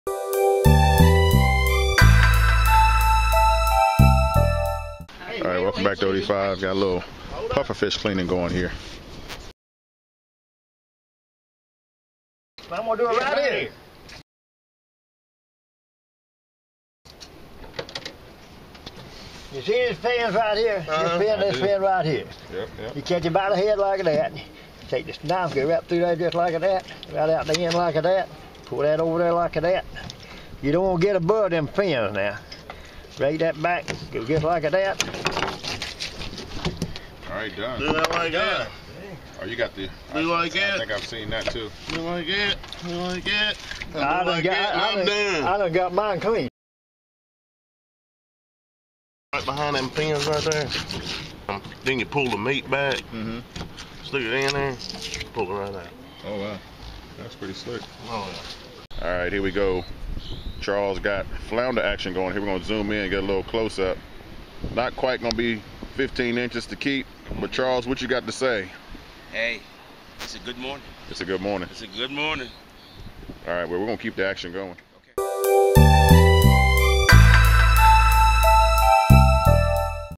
Puffer Fish Cleaning Duel Kure Beach North Carolina
Here is your chance to vote. Which method do you believe is better? These are Northern Puffer fish!!